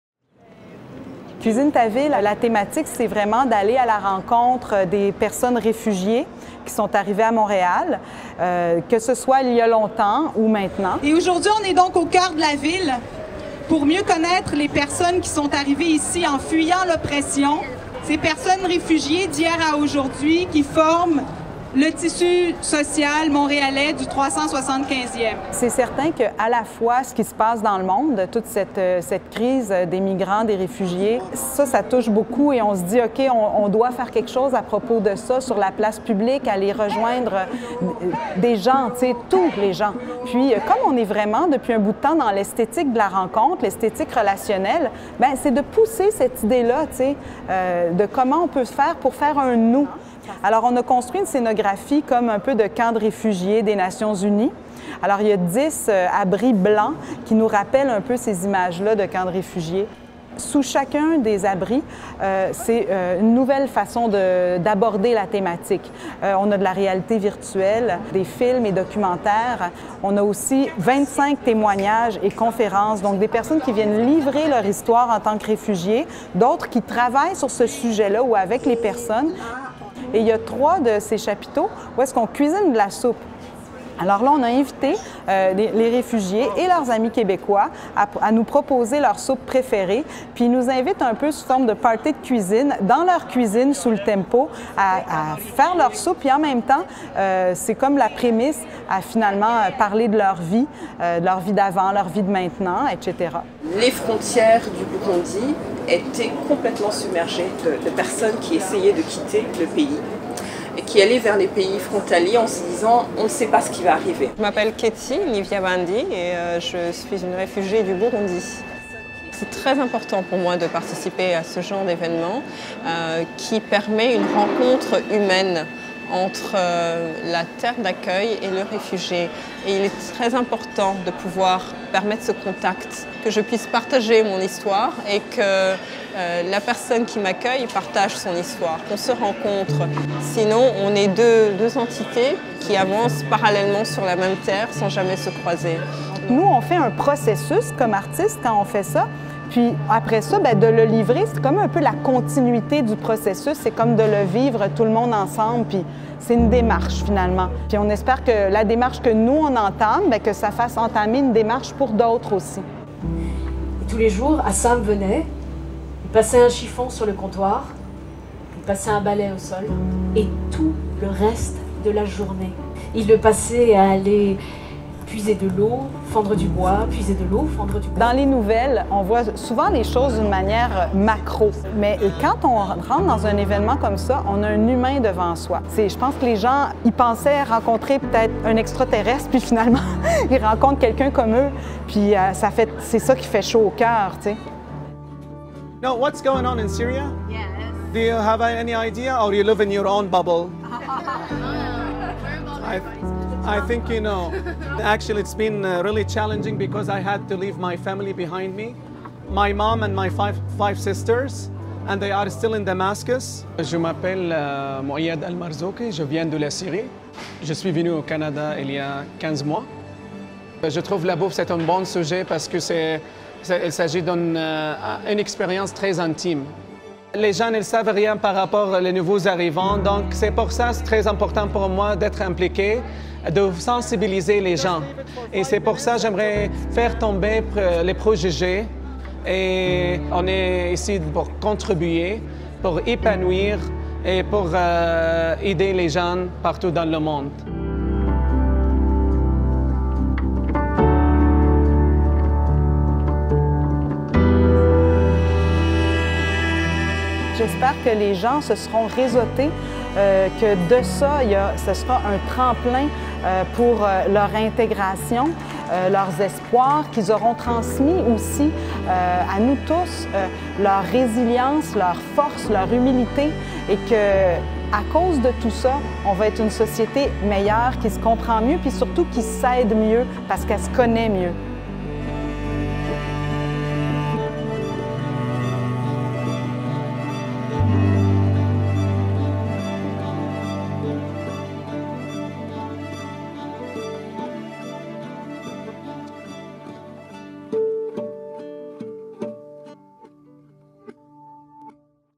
Cuisine Ta Ville ATSA
Un événement d’art relationnel pour mieux connaître les personnes réfugiées d’hier à aujourd’hui qui composent le tissu social montréalais. Cuisine Ta Ville a eu lieu sur l'Esplanade de la Place des Arts du 12 au 14 mai 2017.

Au cœur de la ville, la mise en scène nous immerge dans un monde qui rappelle le périple de la fuite, le souvenir de la maison que l’on a quittée et celle qui nous accueille avec la vie à reconstruire. Une enfilade d’abris de type tempo nous rappelle certains camps de réfugiés. Sous chaque abri, un univers de rencontre spécifique, chaleureux et informant nous est offert.

Il y a ceux qui sont intégrés, ceux qui viennent d’arriver et ceux qui arriveront... Il y a ceux qui accueillent, avec toute l’humanité et les appréhensions que cela peut susciter. Le projet propose une perspective historique, un constat du présent et les enjeux d’intégration à la société d’accueil, une réflexion sur ce qui s’en vient dans le contexte international actuel.